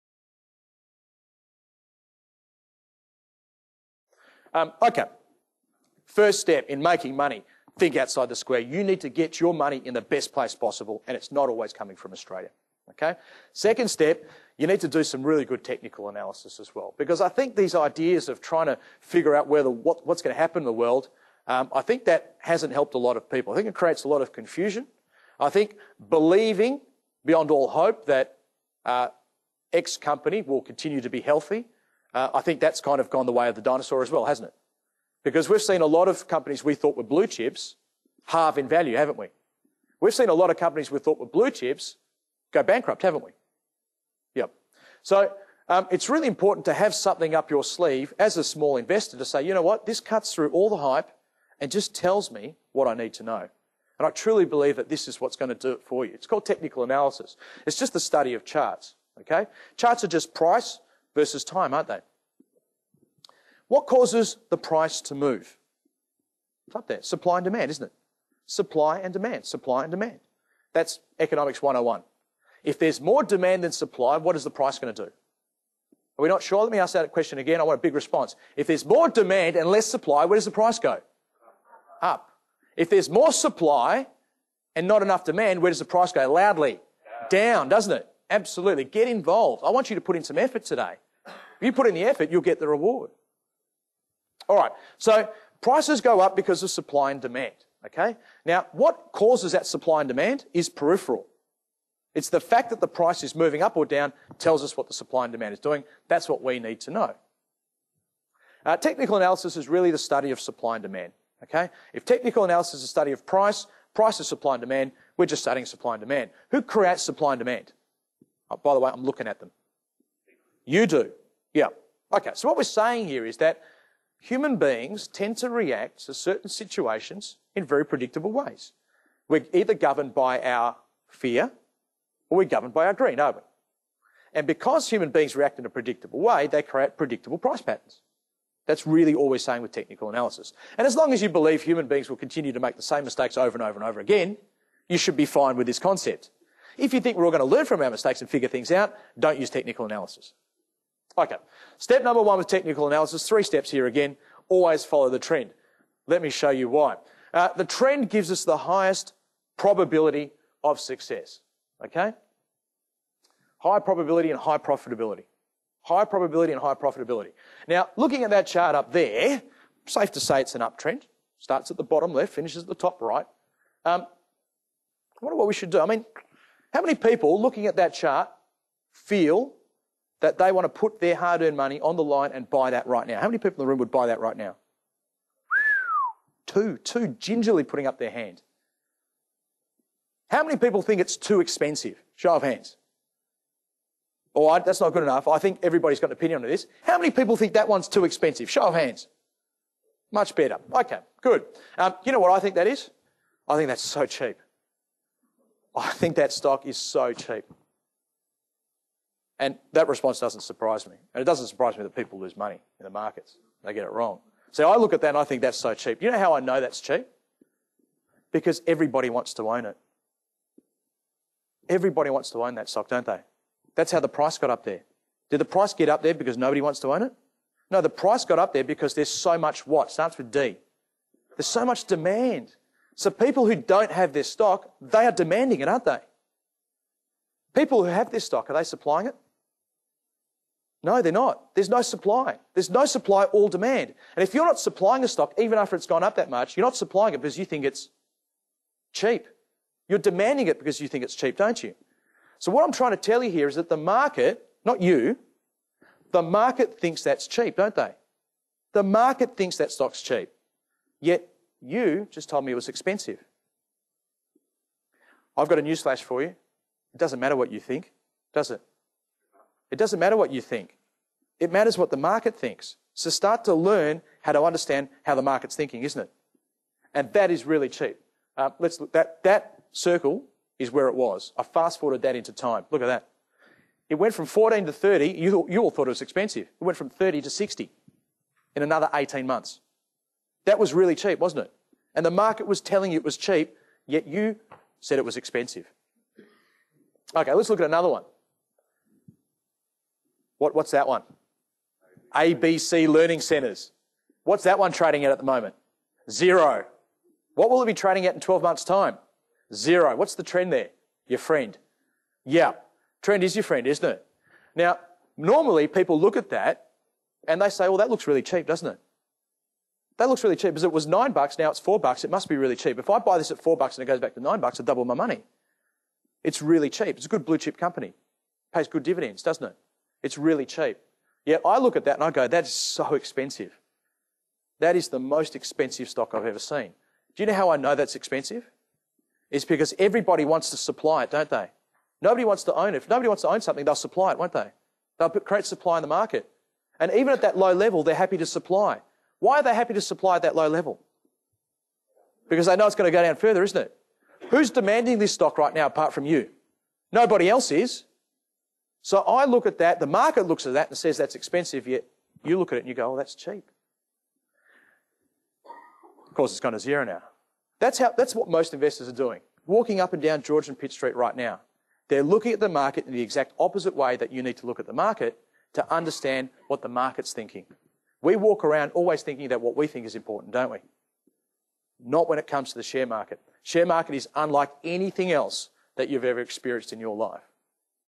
Introduction to Trading CFDs: Part 9/12 Always Follow the Trend
Part 9 of 12 Introduction to Trading CFDs Seminar. Hosted by Carl Capolingua, head of education at Australian Stock Report.